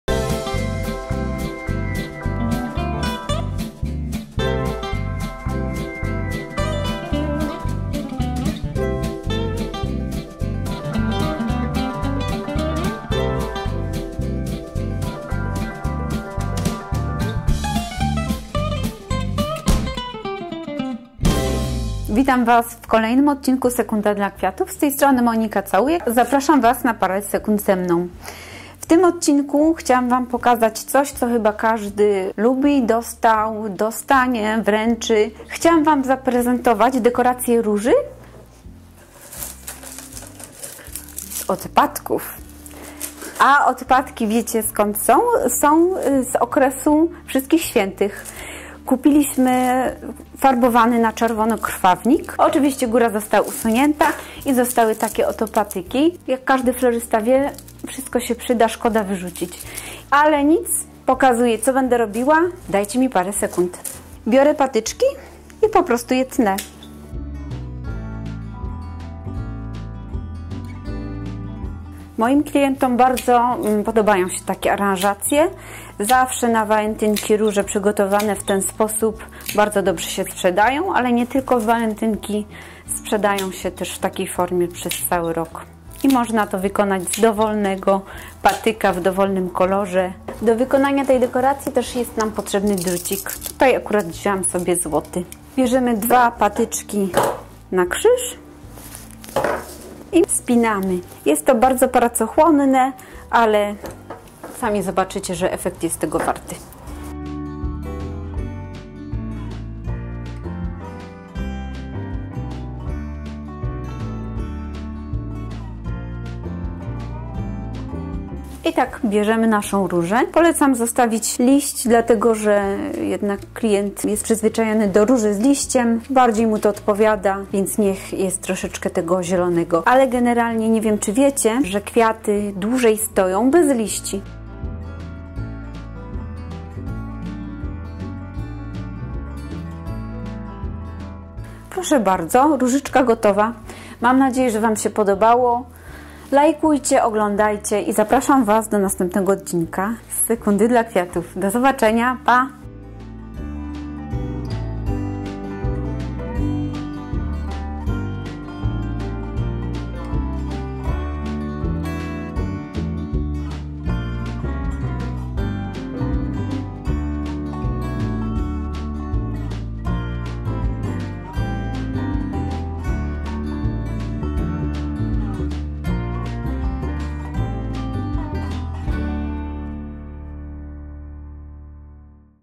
Sekunda dla Kwiatów - ...po prostu RÓŻA - S05 E06 (floristic DIY: just ROSE)
Kolejny odcinek z serii Sekunda dla Kwiatów, jest dla wszystkich z Was, którzy chcielibyście przygotować jakoś pojedynczą róże, ale nie macie pomysłu jak... to już macie! :D

Proste choć wcale nie takie szybkie, ale przyznacie że bardzo efektowne!

Brawa dla Moniki Całujek - która dzieli się z Wami swoim pomysłem.

Zapraszamy
Cesar&Daro

Zajrzyjcie też do naszych innych społecznościówek:
➨ FB i Insta: @miejskiogrodnik